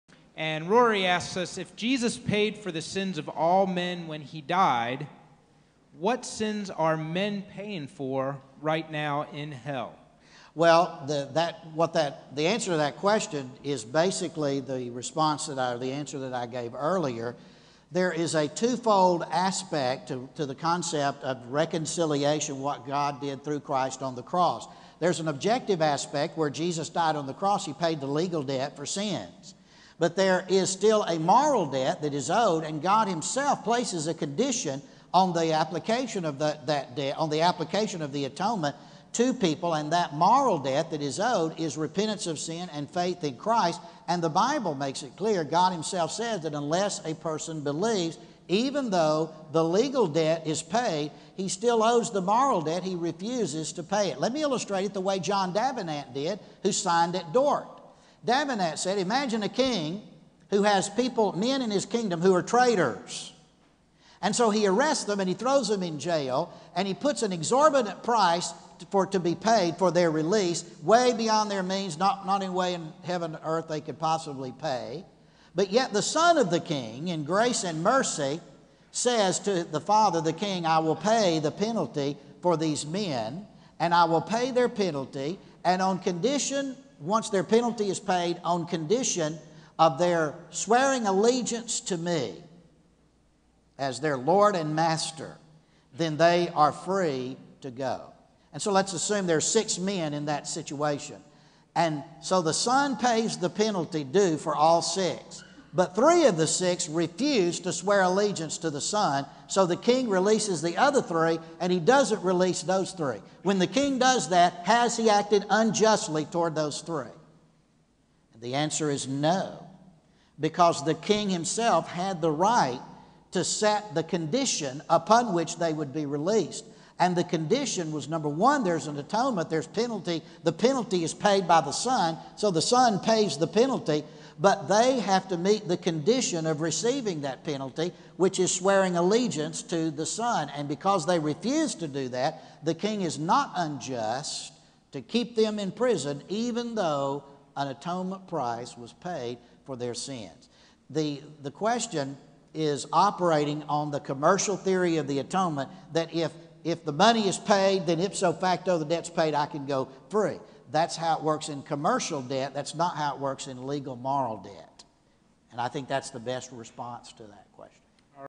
‪Dr. David Allen on the Atonement (Refuting Calvinism's "Double Jeopardy" Argument)‬
On October 1, 2013, Dr. David Allen delivered a lecture at lecture at Liberty University titled, "The Extent of the Atonement and Preaching, Evangelism, and Missions". The full lecture can be found here: ‪